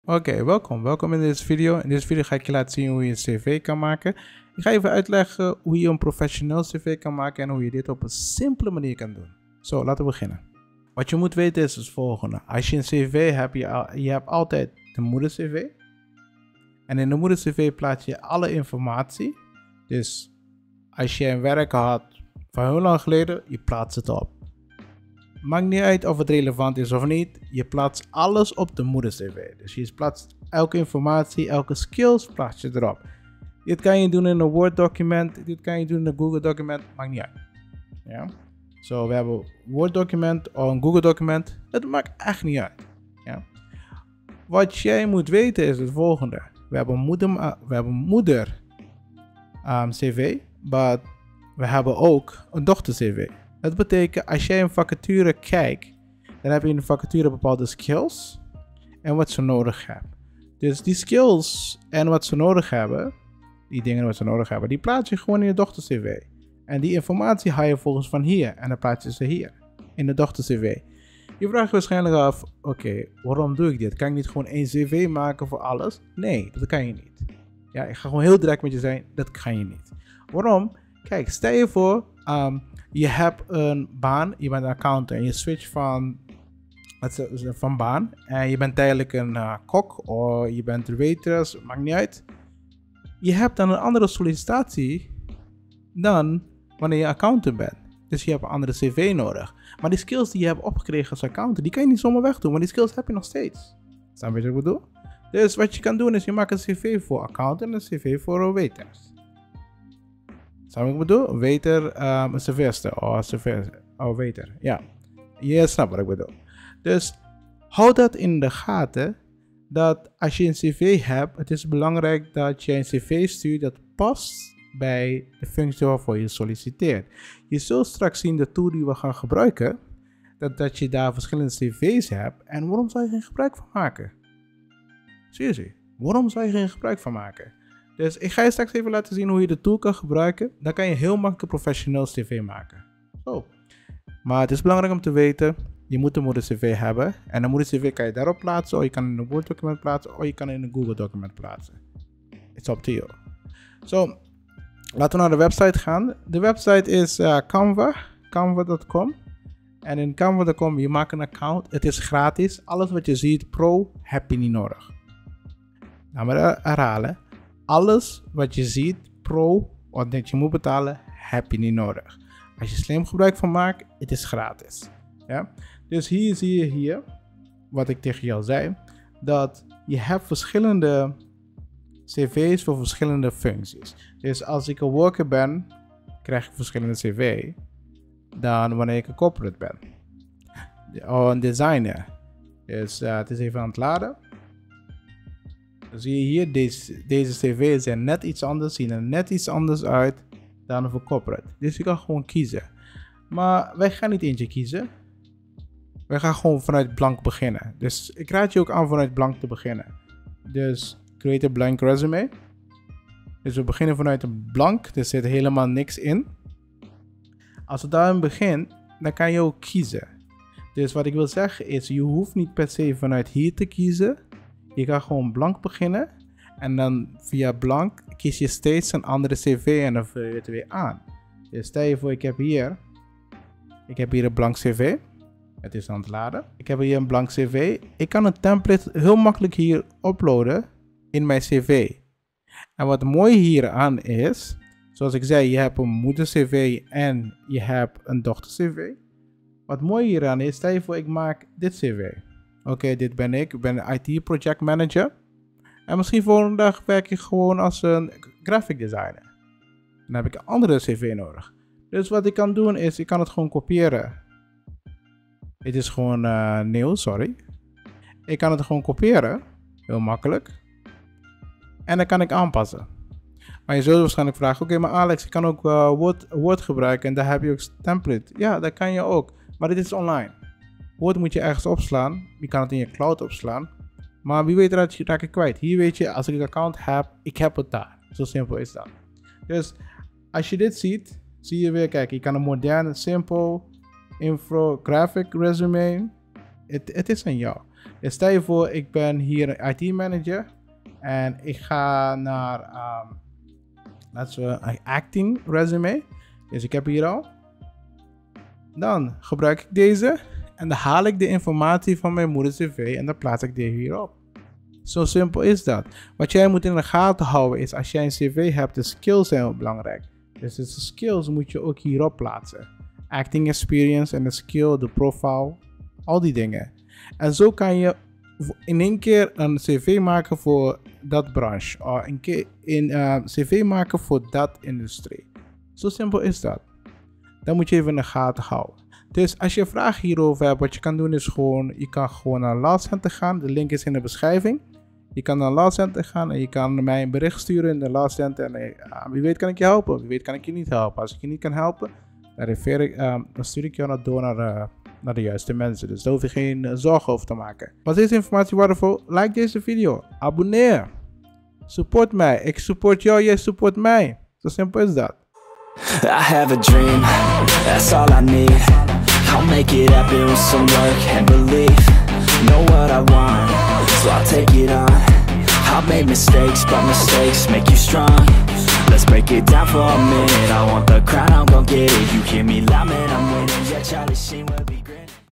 CV maken 2022? Zo doe je dat!
Moet je snel een overzichtelijk cv maken, maar heb je weinig tijd? Of ben je op zoek naar een gratis cv-template? In deze video leg ik je uit hoe je snel een CV kan maken.